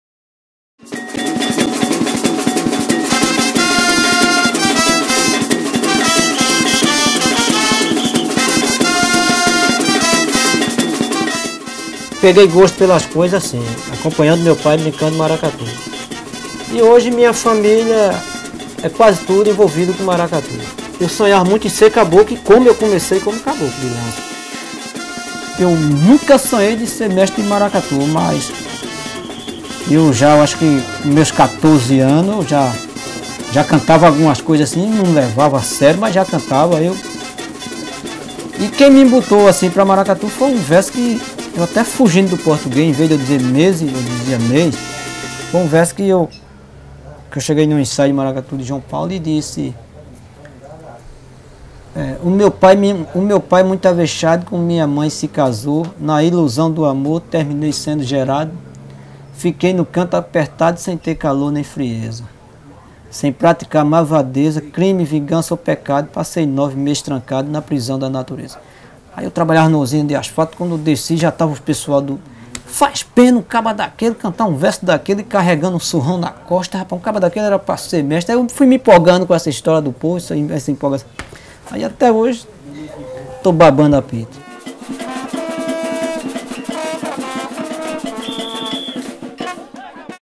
Barachinha conta sua história no Maracatu
O mestre que comandou o histórico Maracatu Estrela Brilhante conta sua história com a cultura popular de Pernambuco e recita o verso que fez ele se tornar uma referência no carnaval. Uma produção da Símio em parceria com a TV Pernambuco.